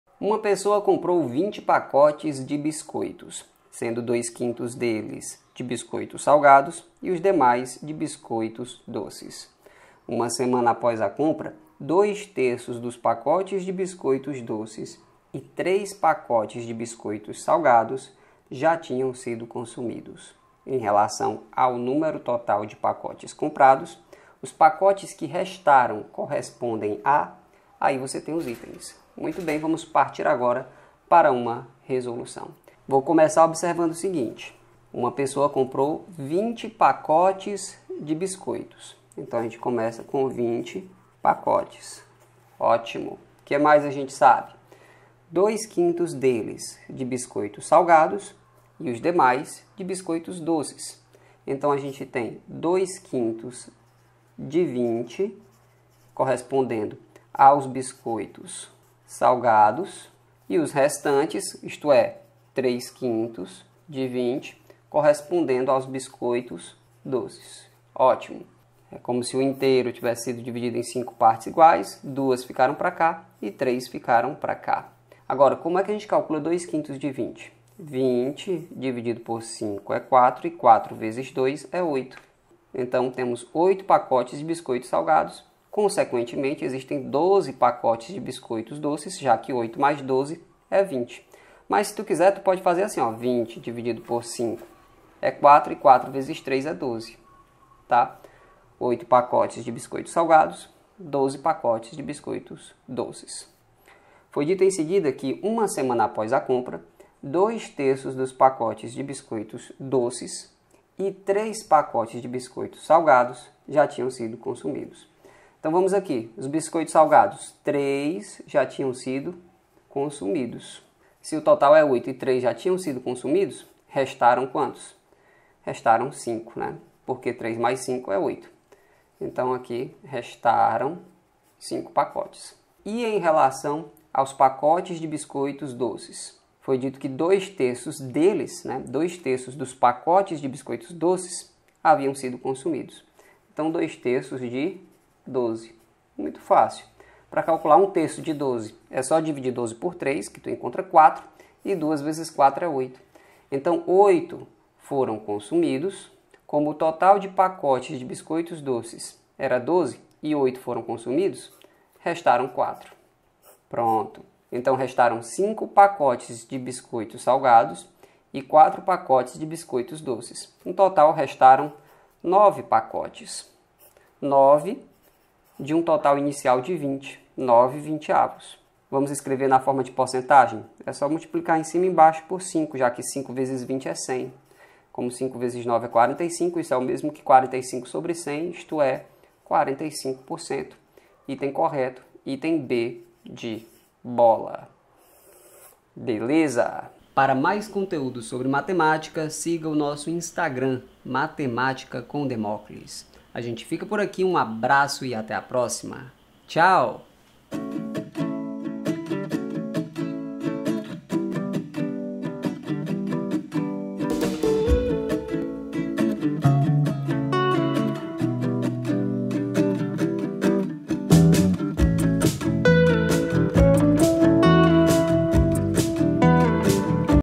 FRAÇÃO DE UMA QUANTIDADE E PORCENTAGEM
Olá, amigos do meu canal no YouTube. Eu sou o professor Demóclis Rocha e, neste vídeo, apresentarei uma resolução para a seguinte questão

16. Uma pessoa comprou 20 pacotes de biscoitos, sendo 2/5
deles de biscoitos salgados e os demais de biscoitos
doces. Uma semana após a compra, 2/3 dos pacotes de
biscoitos doces e 3 pacotes de biscoitos salgados já
tinham sido consumidos. Em relação ao número total de
pacotes comprados, os pacotes que restaram correspondem a

(A) 40%.
(B) 45%.
(D) 55%.
(E) 60%.

CÂMERAS

MICROFONES